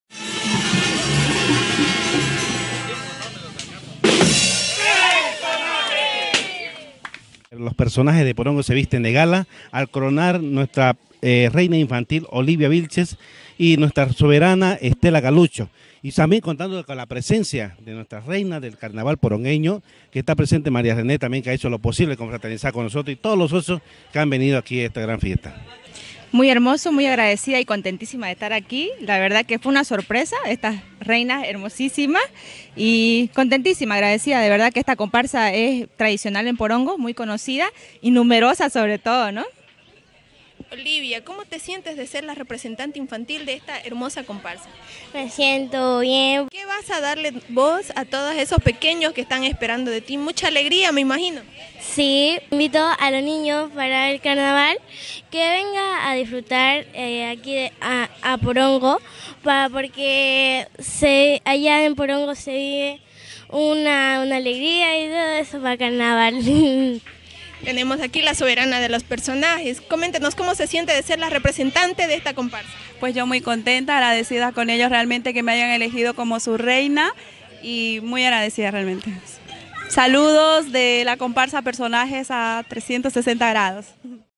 COMPARSA LOS PERSONAJES DE PORONGO
PROCLAMACIÓN DE LAS REINAS DE LA COMPARSA LOS PERSONAJES DE PORONGO